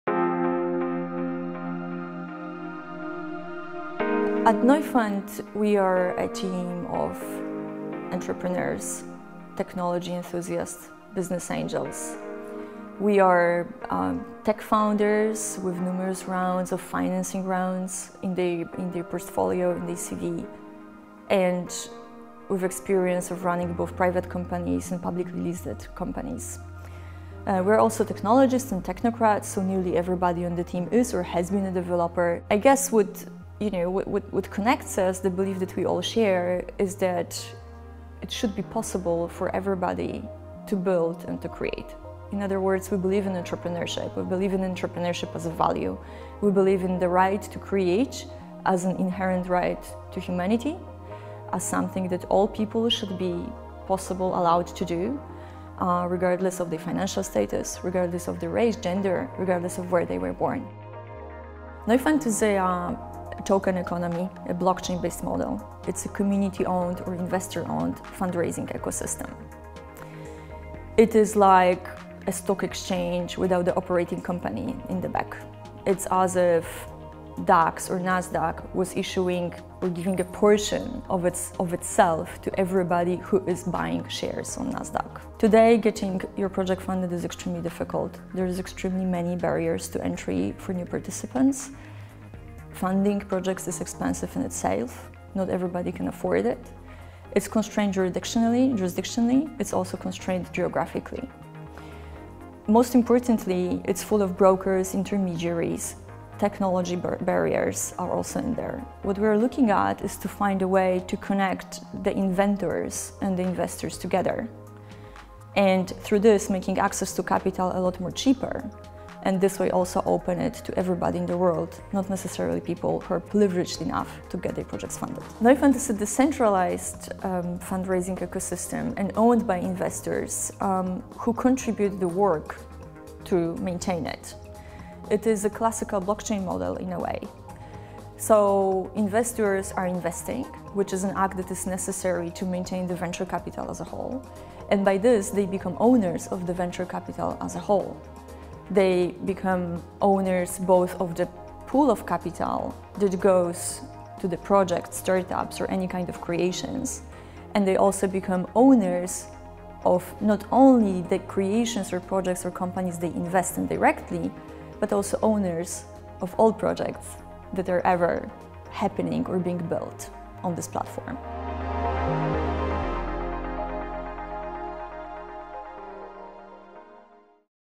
Zoe Adamovicz - How Neufund reinvents the way we fund companies
Neufund is a community-owned or investors-owned fundraising platform bridging the worlds of venture capital and blockchain space.

To learn more about Neufund and its upcoming ICBM go or join our Telegram chat